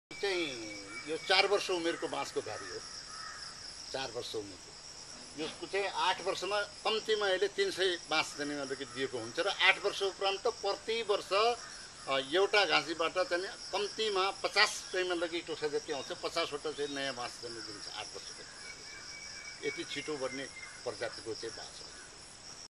Yield of Bamboo
Bamboo is fast growing species so, yield cam be acquired from bamboo plantation in shorter time than other trees species.
Mr. Dev Datta, Kanchanpur, Nepal